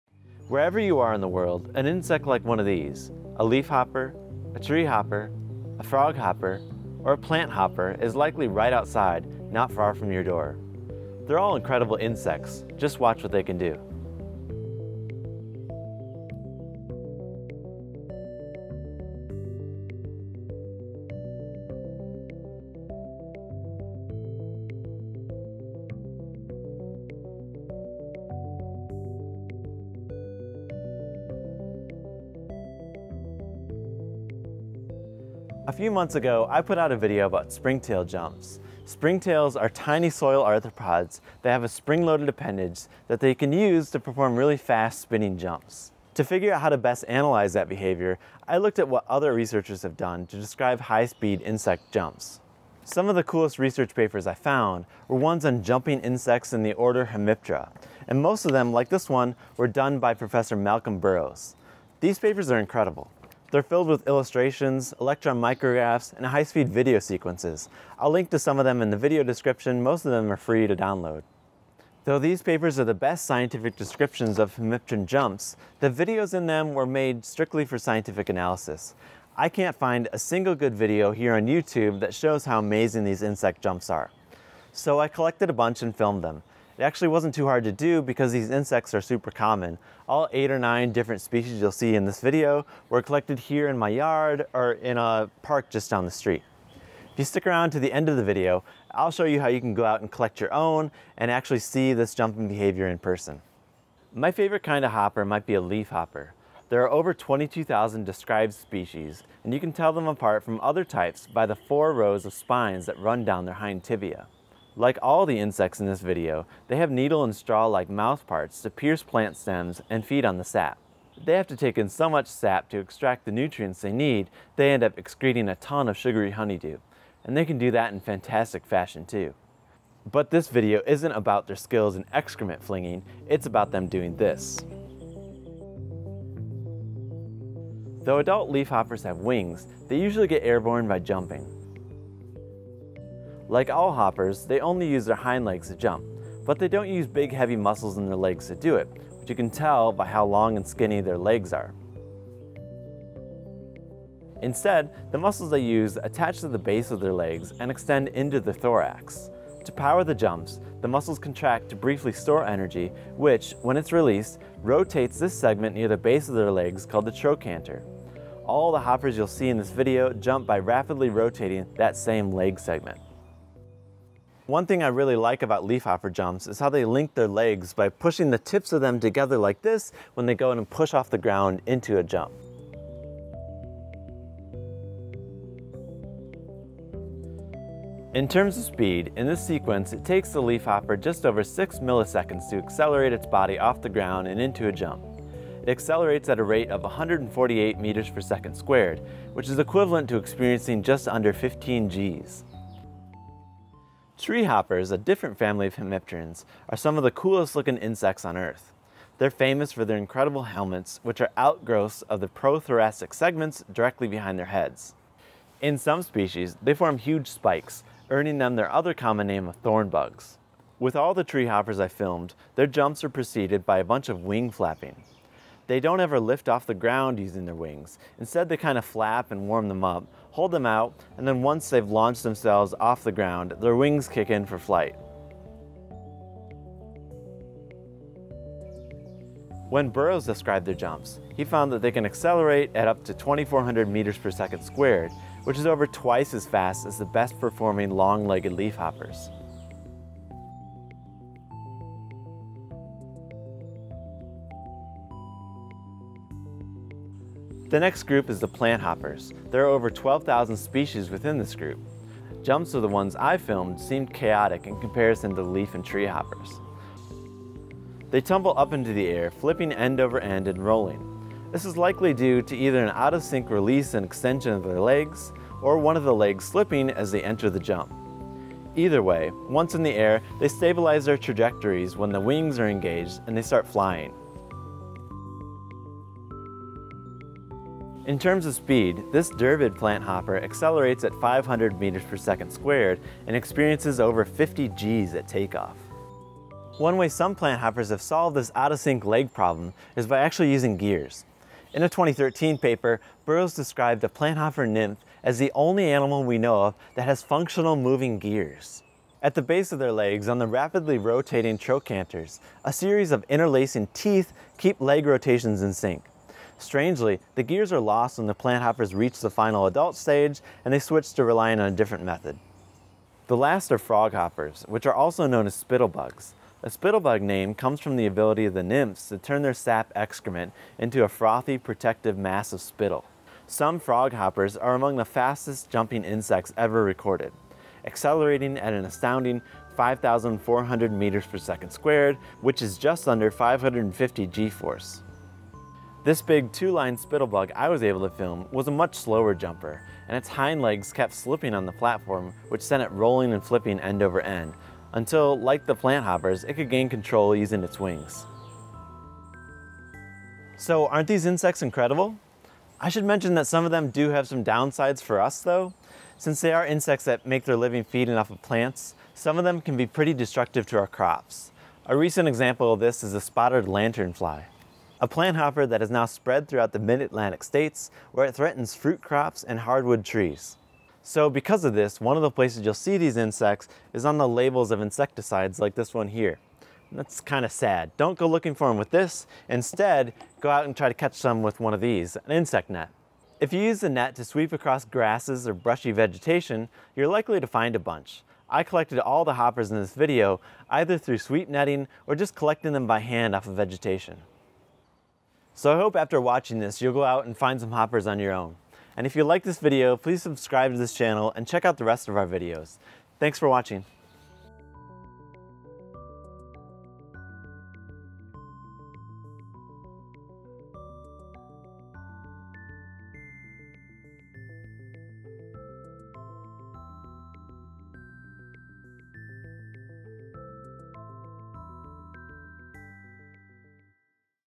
Jumping Hoppers in SLOW MOTION!
Leafhoppers, treehoppers, planthoppers, and froghoppers are all insects in the order Hemiptera that are some of the fastest jumping animals on earth! They are also super-common, so I collected a bunch and filmed their jumps in slow-motion!

References (Prof. Malcolm Burrows papers):
Froghopper jumps - Burrows M (2006), “Jumping performance of froghopper insects.” J Exp Biol 209:4607-21

Froghopper gears - Burrows M, Sutton G (2013), “Interacting gears synchronize propulsive leg movements in a jumping insect.” Science 341(6151):1254-6

Treehopper jumps - Burrows M (2013), “Jumping mechanisms of treehopper insects (Hemiptera, Auchenorrhyncha, Membracidae).” J Exp Biol 216:788-99

Planthopper jumps - Burrows M (2009), “Jumping performance of planthoppers (Hemiptera, Issidae).” J Exp Biol 212(17):2844-55

Leafhopper jumps - Burrows M (2007), “Kinematics of jumping in leafhopper insects (Hemiptera, Auchenorrhyncha, Cicadellidae).” J Exp Biol 210:3579-89